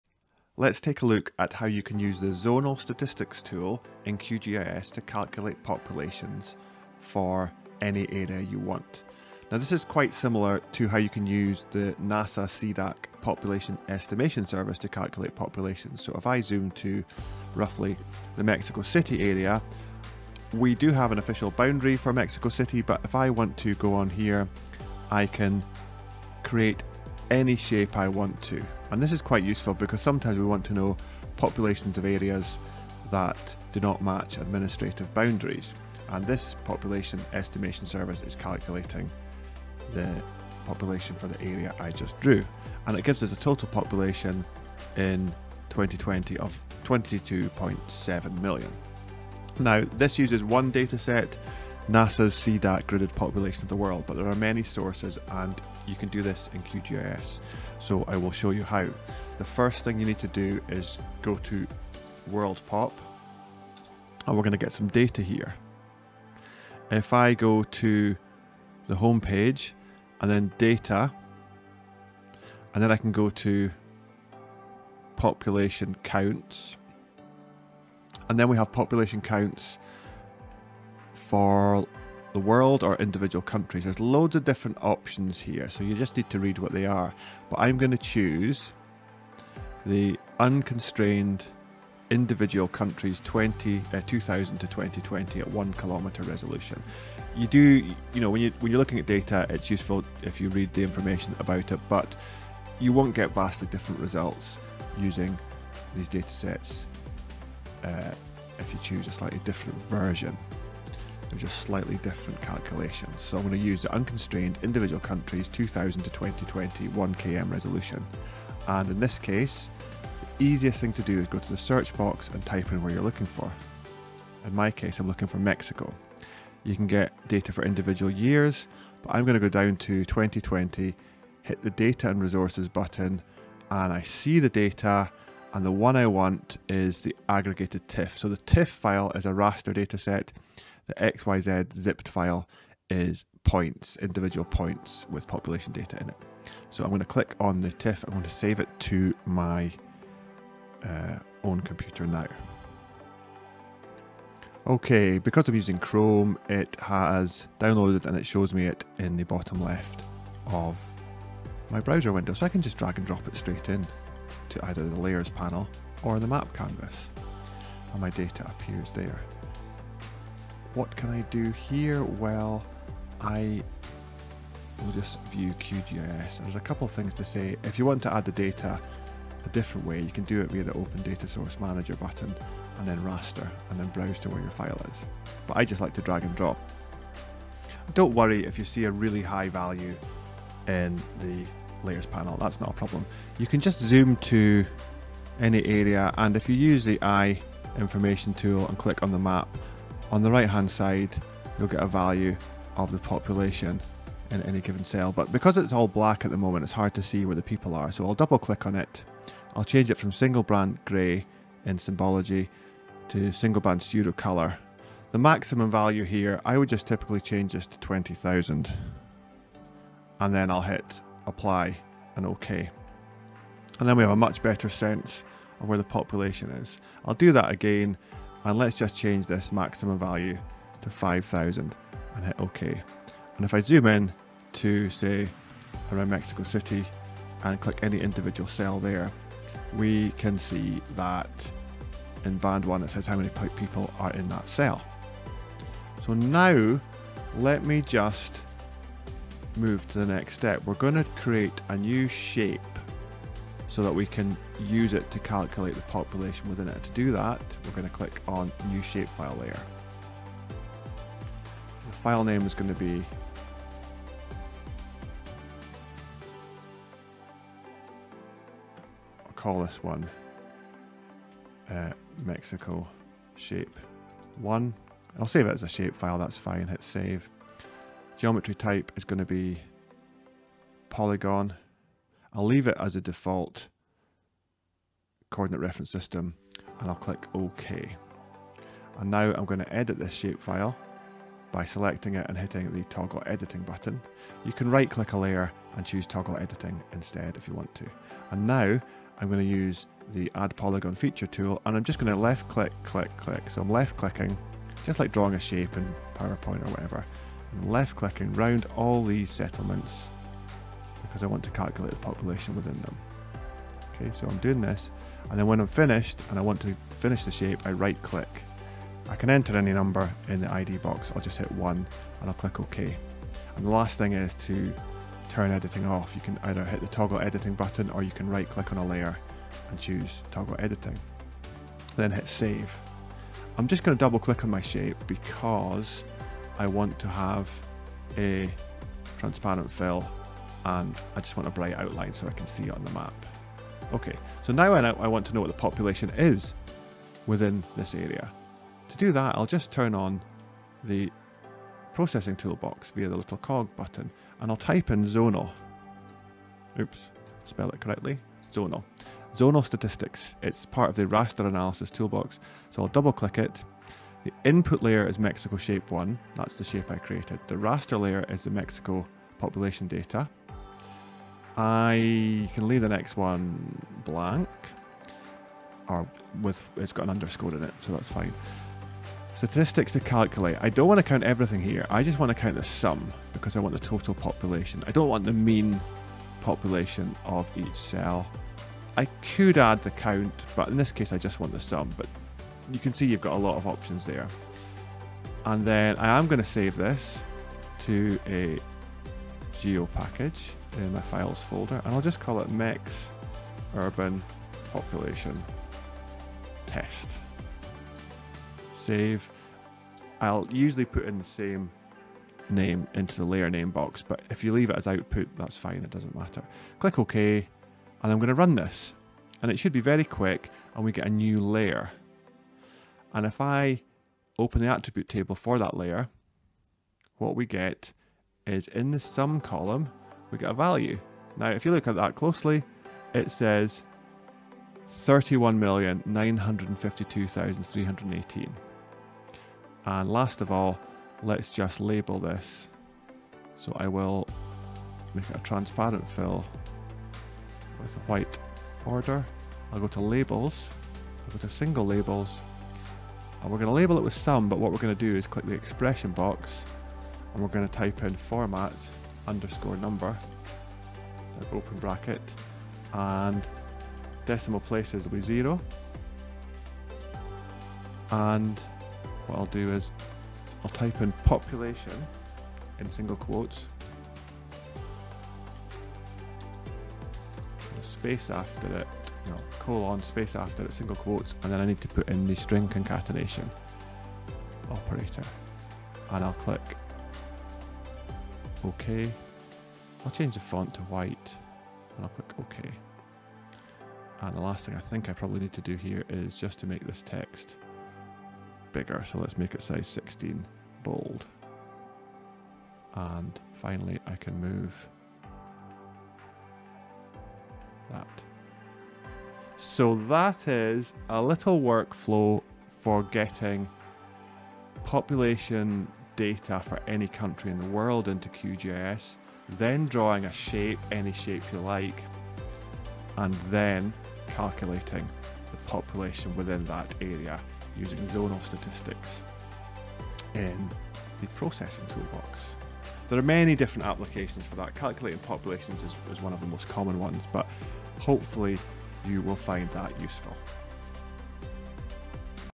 Calculate populations for anywhere in QGIS
In this video I demonstrate a workflow of how you can calculate the population for any area in the world using global raster population data from WorldPop. In the example we do this for an area of Mexico but you can find data for your own country - or the entire world - and replicate it very easily. If you already have your own shape then you don't even need to do the vector layer creation bit included here.